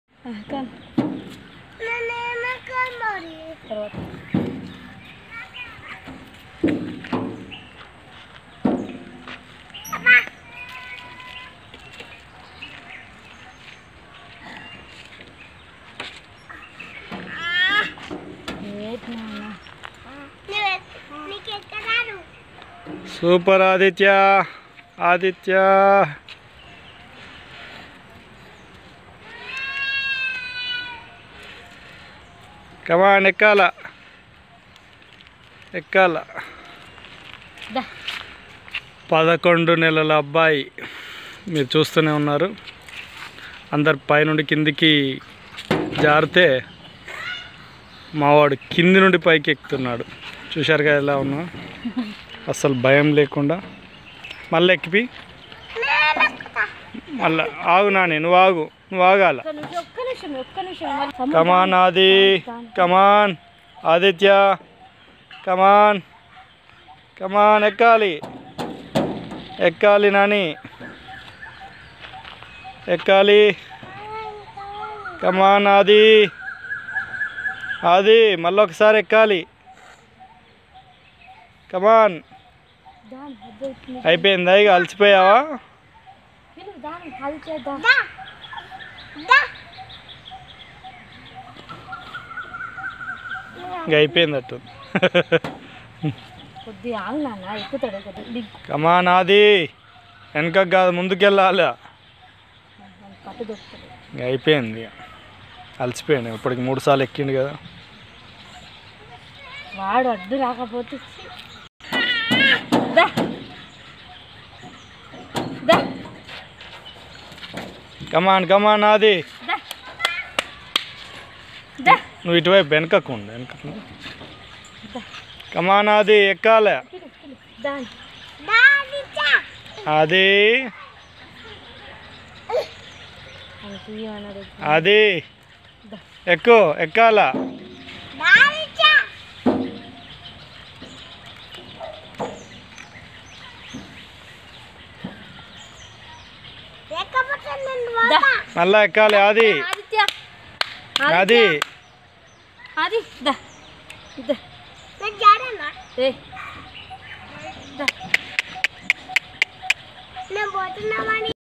వండర్ చైల్డ్ ఆదిత్య | You Will Shock After Watching this Video | Wonder Kid Aditya | Naveen Valloju
వండర్ చైల్డ్ ఆదిత్య | You Will Shock After Watching this Video | Wonder Kid Aditya | Naveen Kumar Valloju

గండర గండడు | గట్టి పిండం - How a 11 months baby climbing a baby slide without any support or fear

at Nandana Vanam, Forest office road, BTS, Nalgonda